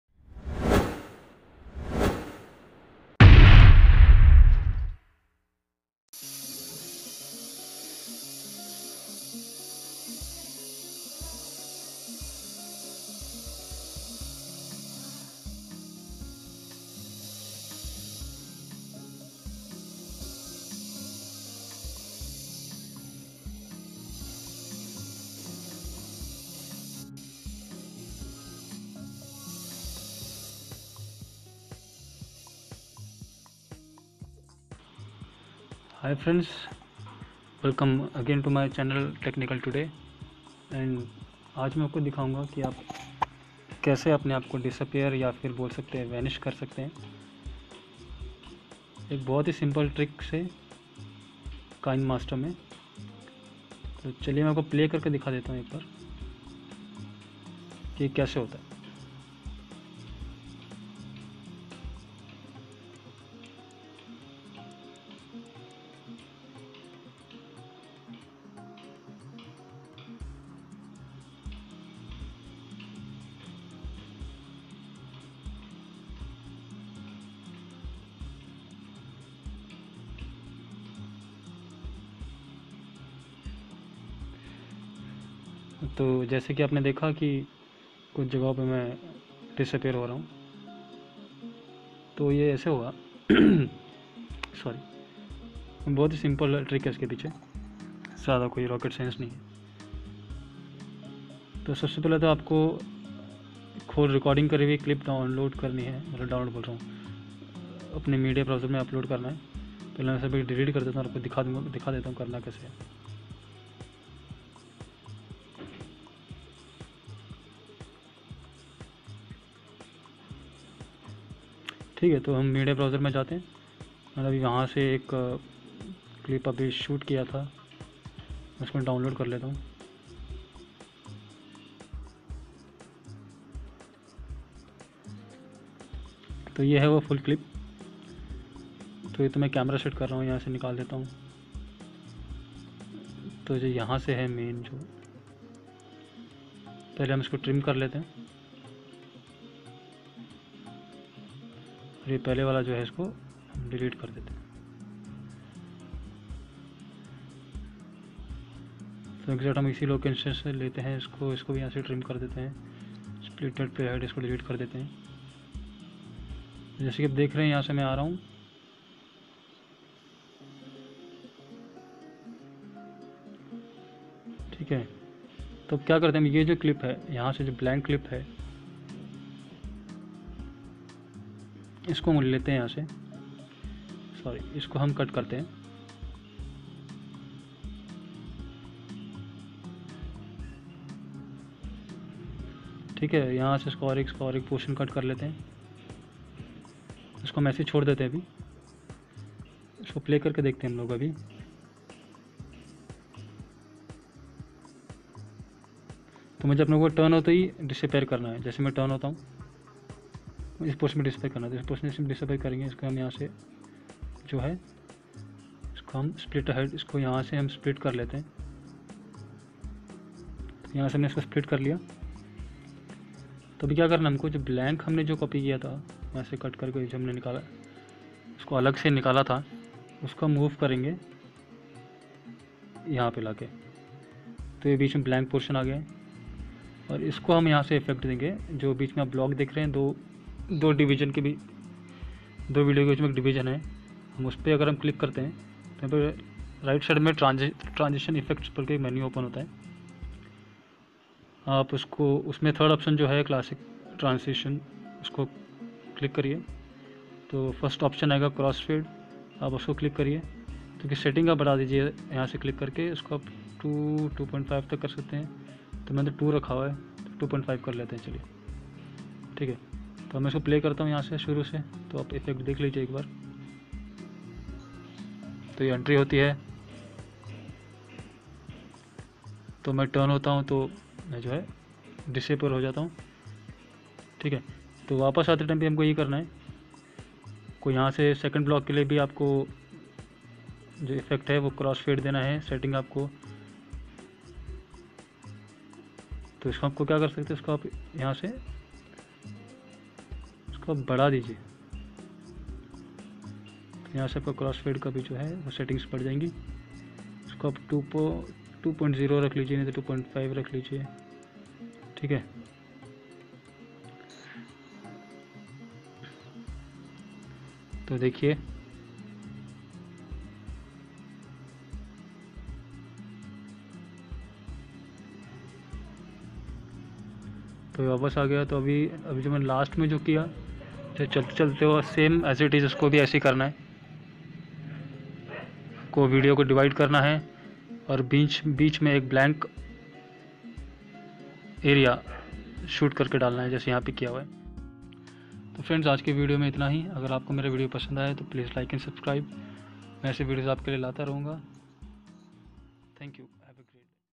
Create vanish effect on mobile || kinemaster tutorial || खुद को गायब कैसे करें
Another Kinemaster tutorial showing how can you disappear and reappear yourself in Kinemaster see full video and do by your self very easy to do and show your friends